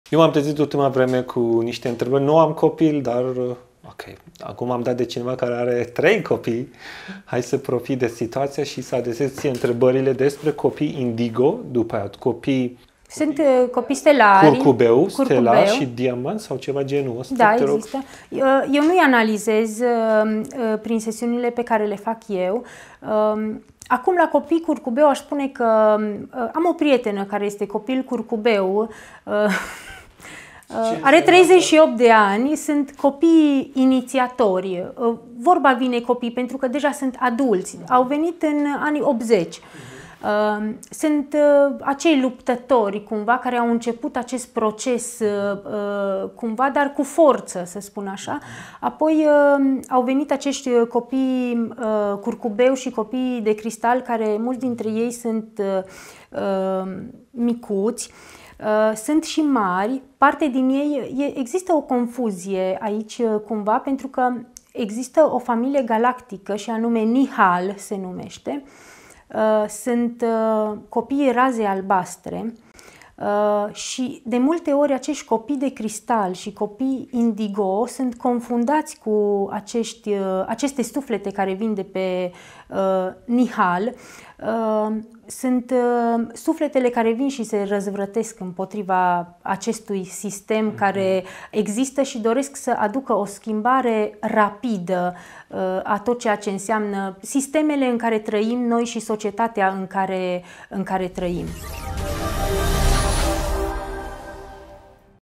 3 generații de suflete care au venit după războiul mondial pe pământ copii Indigo, Cristal, Curcubeu
1️⃣ Copiii indigo, sunt primul val de suflete voluntare care au venit pe pământ după războiul mondial în anii '50 '60. Pentru ei era cel mai greu prezența și existența și viața pe pământ în perioada aceea când vibrația pământului era foarte jos.
Mulți dintre ei alegeau se sinucide din cauză că nu mai rezistau la vibrația la atâta ură dușmănie sânge și negativitate.

2️⃣ Copiii cristal sunt al 2-lea val de suflete voluntare care au venit pe pământ, născuți în anii '80.
Au venit din diferite stele, galaxii, universuri și dimensiuni pt a ridica vibrația planetară.

3️⃣ Copiii curcubeul sunt al treilea val de Suflete Voluntari care au venit pe pământ după războiul mondial și sunt născuți după anii '90.

Ei vin cu vibrația Păcii în sus. Bazat pe tabelul conștiinței vibrația 600 (pace) care influențează pozitiv peste 10 milioane de oameni în jurul lor.

O parte din aceste suflete voluntare vin din Nihal, sunt copii razei albastre, ei vin să răzvrătesc împotriva acestui sistem și doresc să aducă schimbare rapidă.

De multe ori copii Indigo și Copii Cristal sunt confundați cu aceste suflete din Nihal.